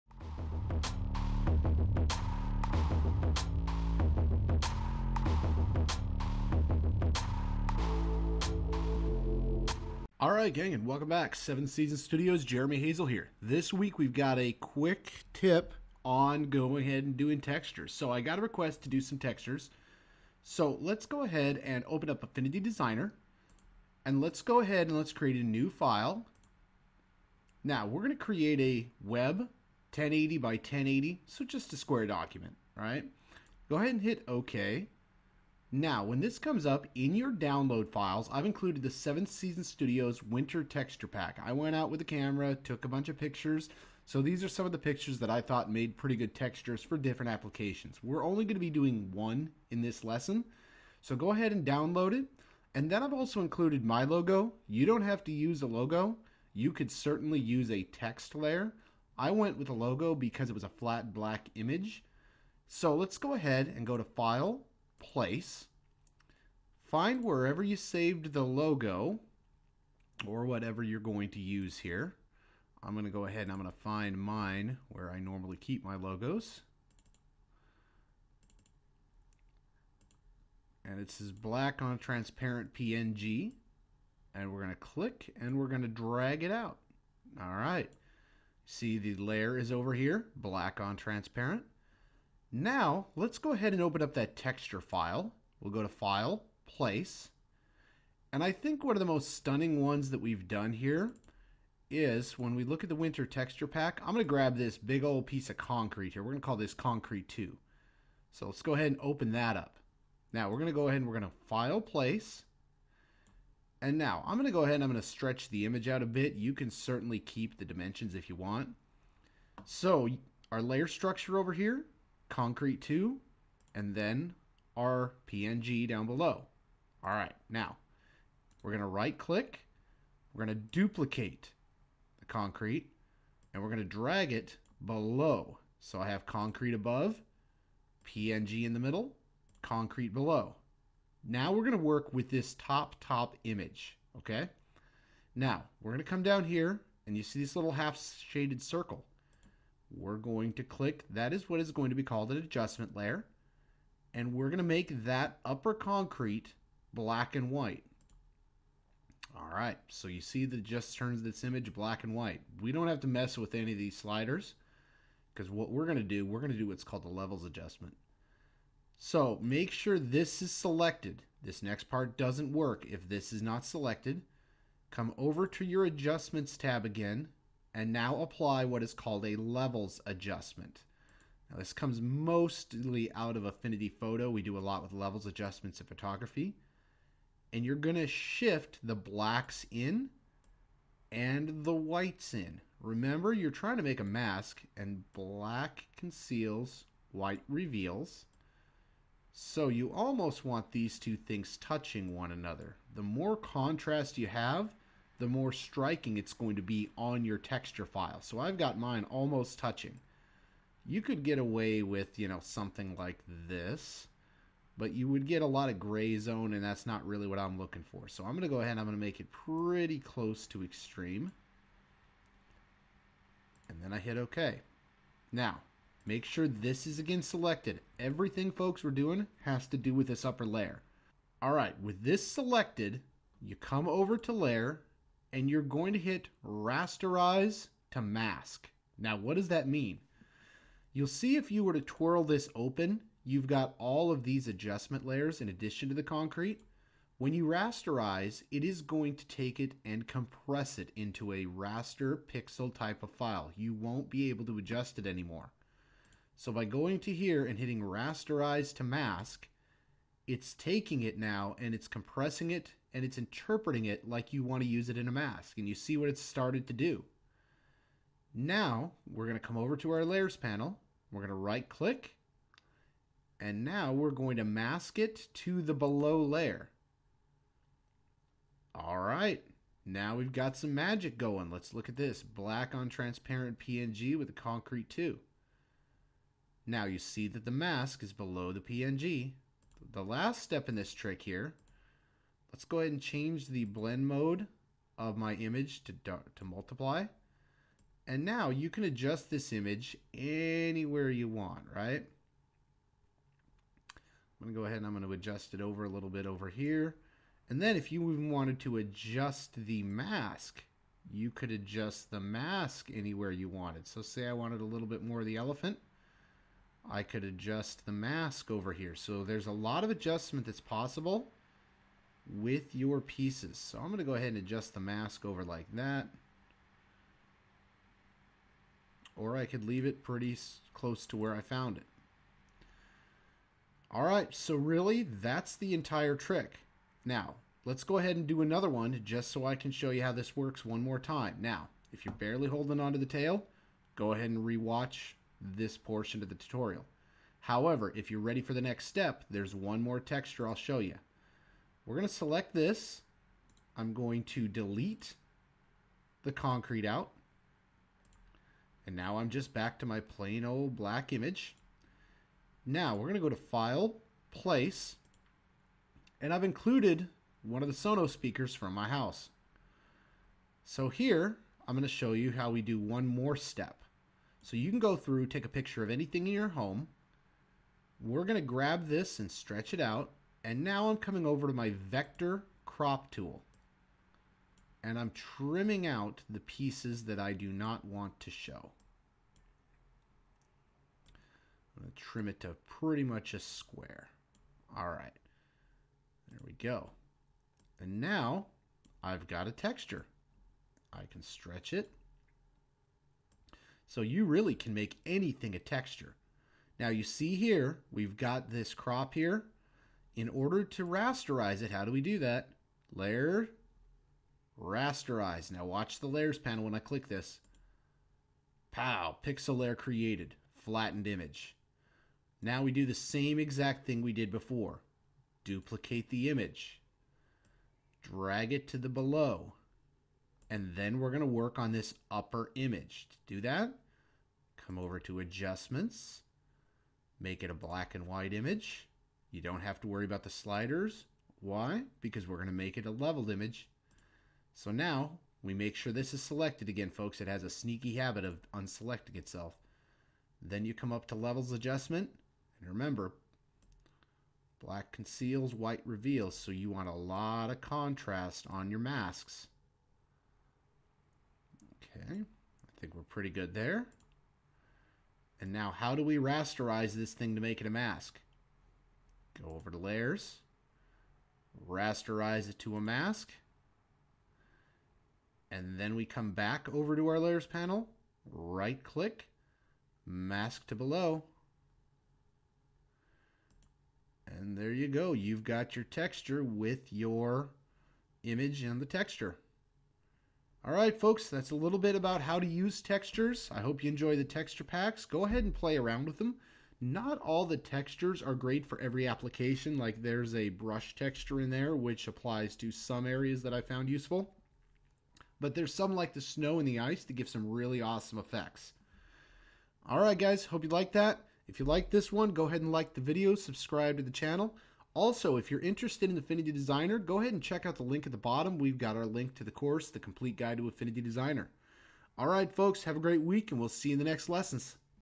Using textures in Affinity Designer -Plus FREE TEXTURE PACK
A simple method to use textures in Affinity Designer
Interested in learning more about Affinity Designer?
Get the entire Affinity Designer Course here at the Youtube viewer discount